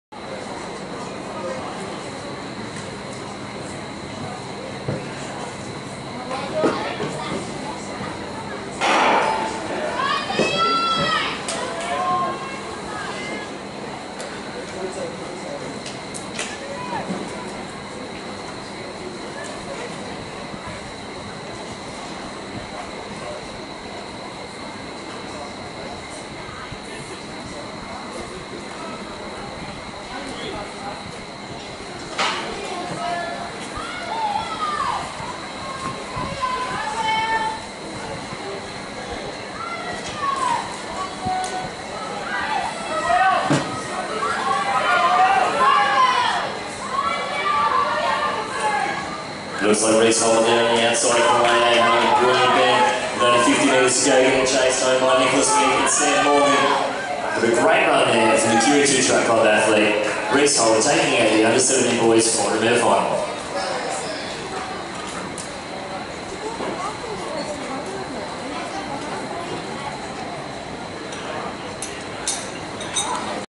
400m U17yrs Boys Final, Queensland Junior Athletics Championships, QSAC 23/02/2017
1st. Reece Holder 50.02,  2nd. Nicholas Meek 50.66,  3rd. Sam Morgan 50.85,  4th. Taige Hooper 51.82.